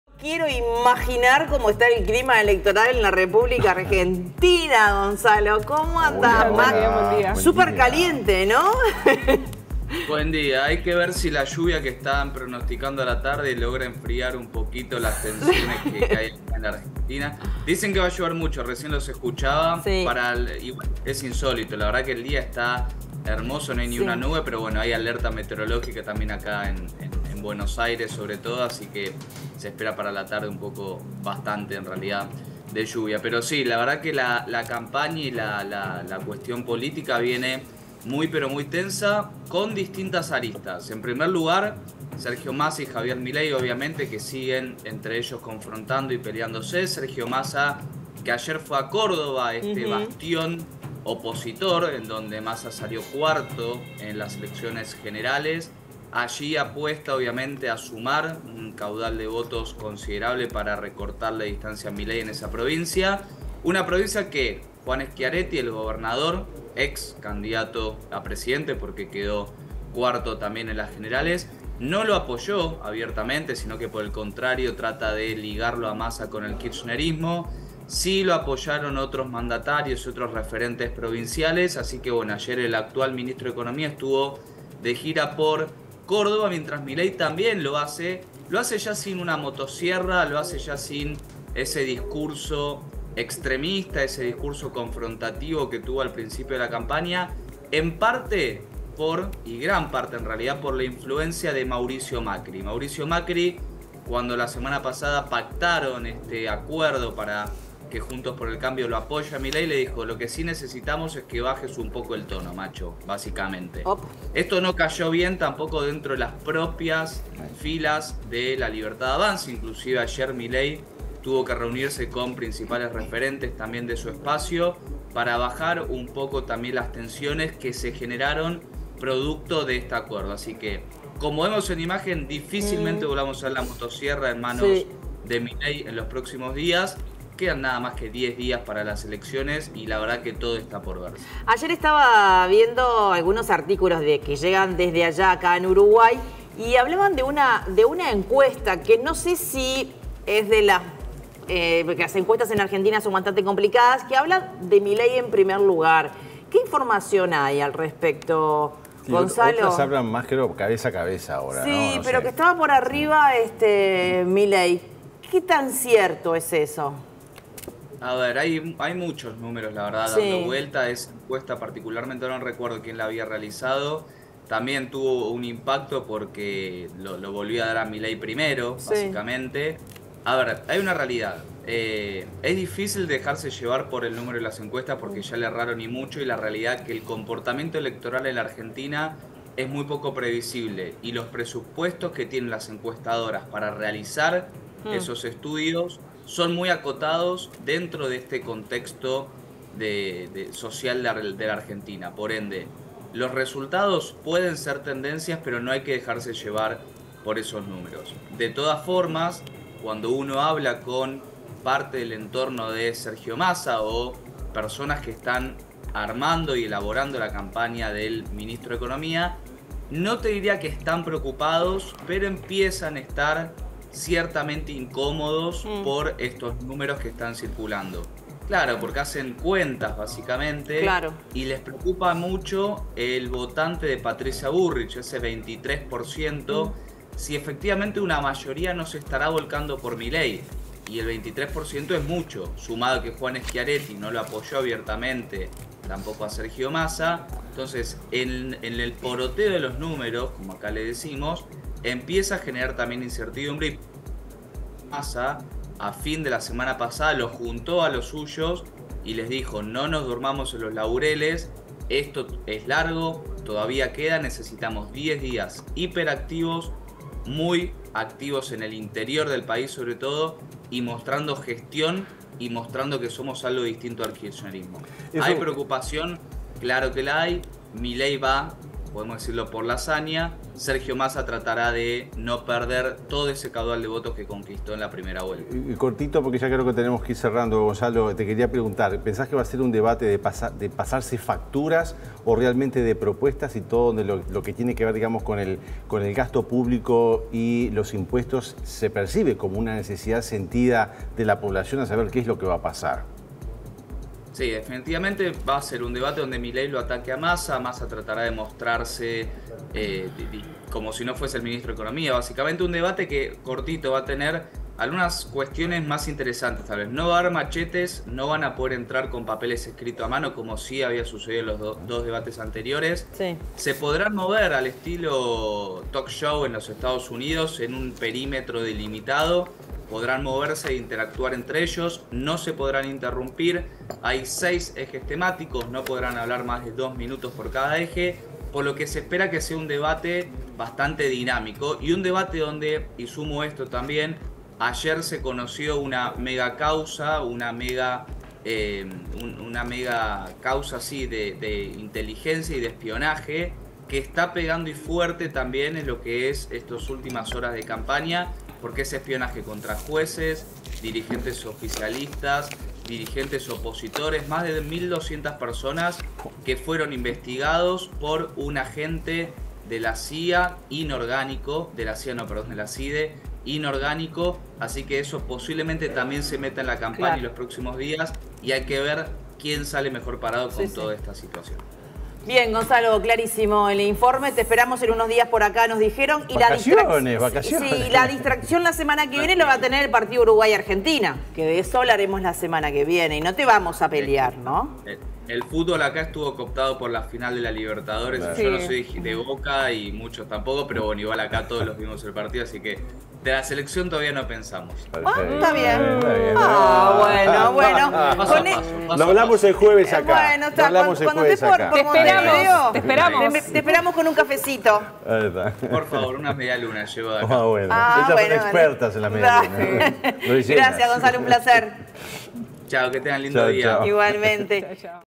8AM - El domingo habrá debate presidencialArgentina8AM 07112023
Recibimos al periodista argentino Gonzalo Bañez con toda la información desde Buenos Aires.